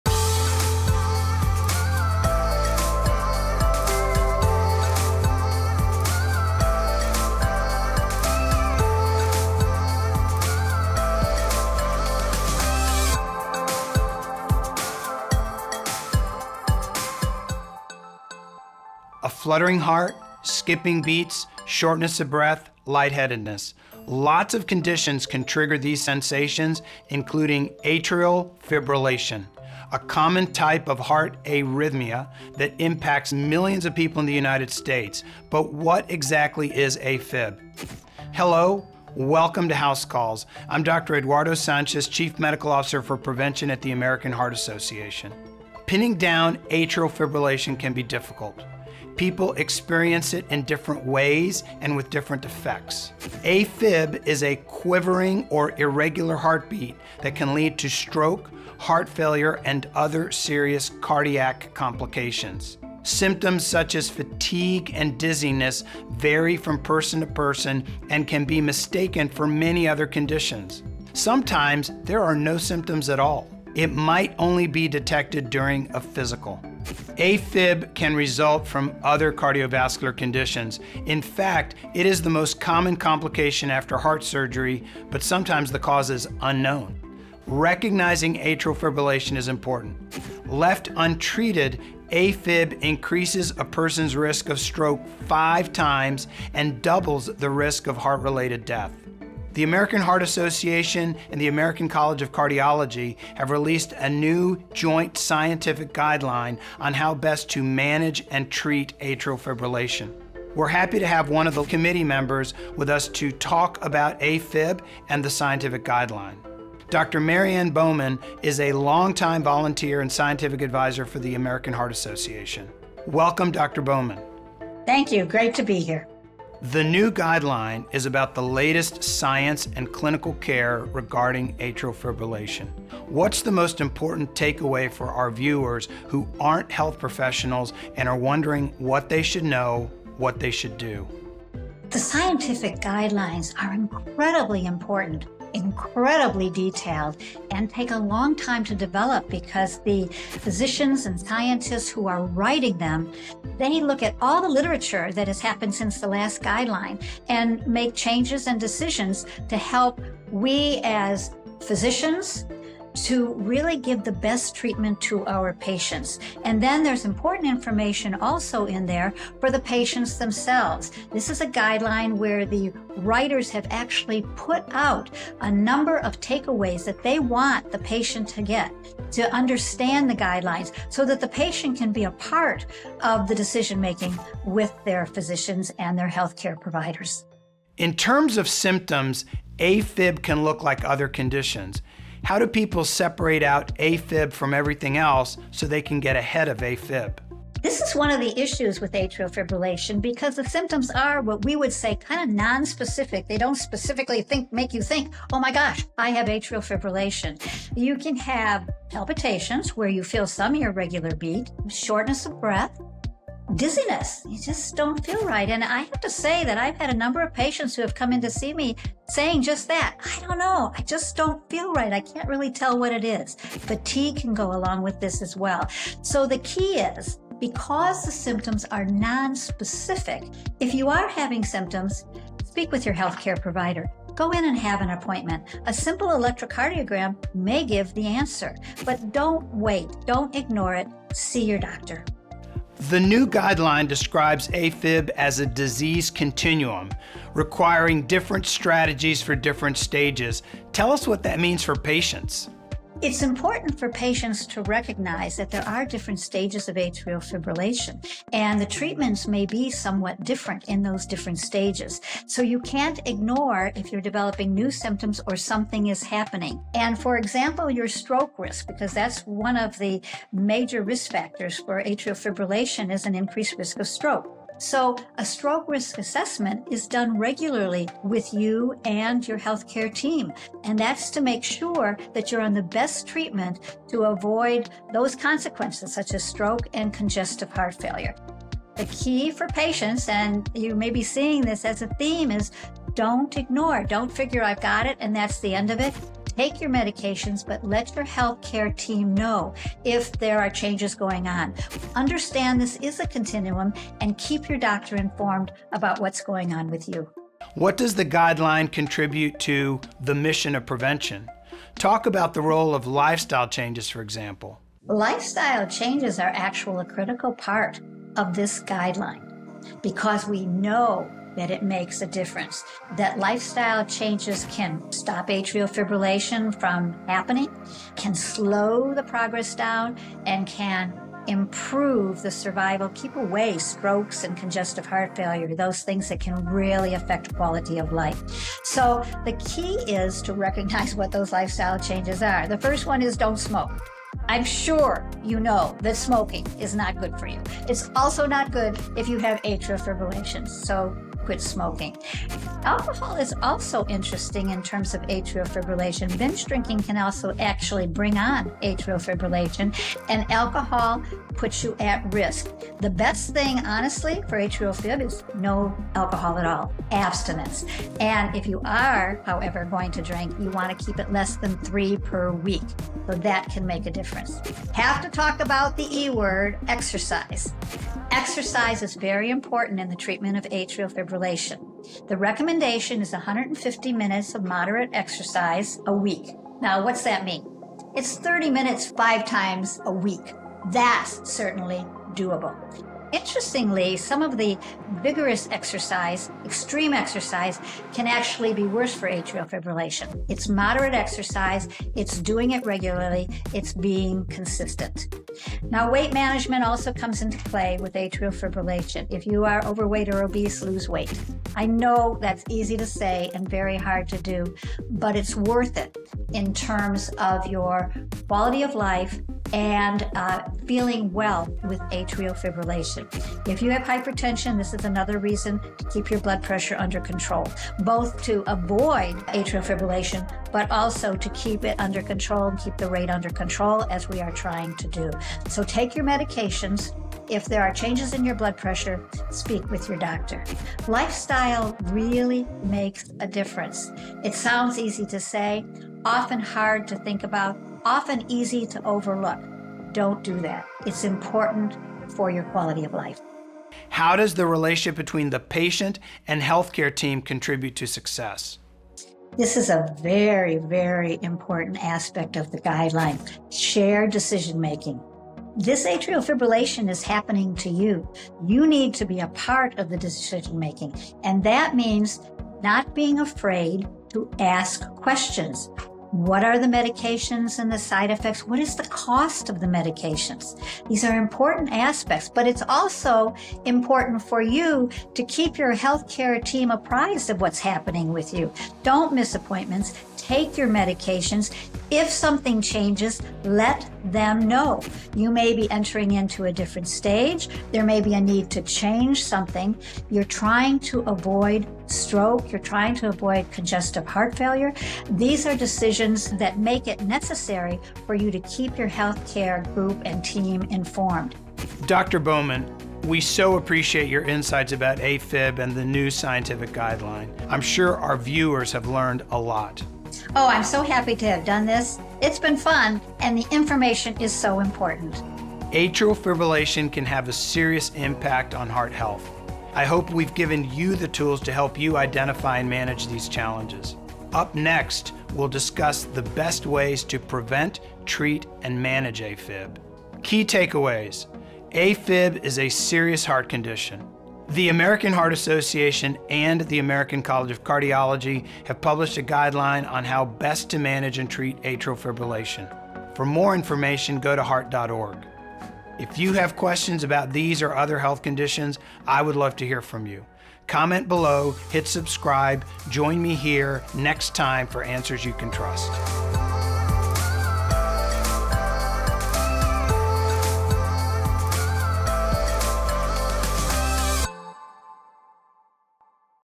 AFib explained: the heart condition you can't ignore
Atrial fibrillation, sometimes called AFib, is a common type of heart arrhythmia that impacts millions of people in the United States, AFib can lead to stroke, heart failure, and other serious cardiac complications.

Dr. Mary Ann Bauman joins Dr. Eduardo Sanchez this week to share what you should know about atrial fibrillation.

0:00 Welcome to House Calls
2:03 The new AFib guidelines
3:07 Recognizing AFib
4:02 Stages of atrial fibrillation
5:07 Lifestyle changes to prevent AFib
7:33 Working with the health care team
8:45 Key Takeaways